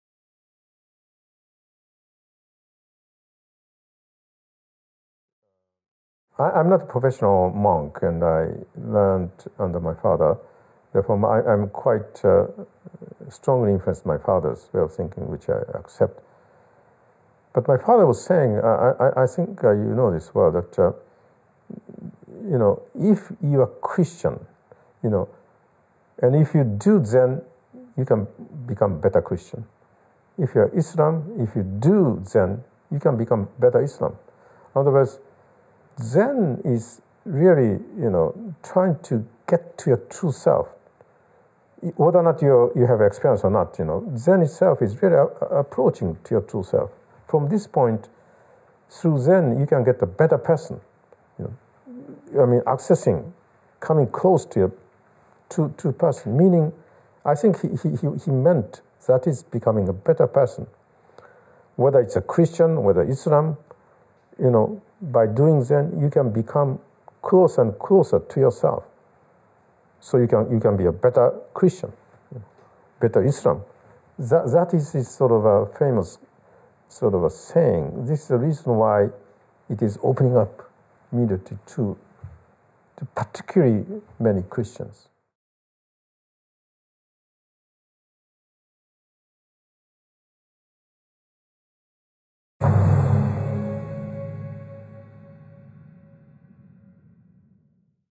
Interview mit Yamada Roshi: What happens to people with a religion, if they practice Zen?
Interview mit Yamada Roshi: What happens to people with a religion e.g. Christianity or Islam, if they practice Zen?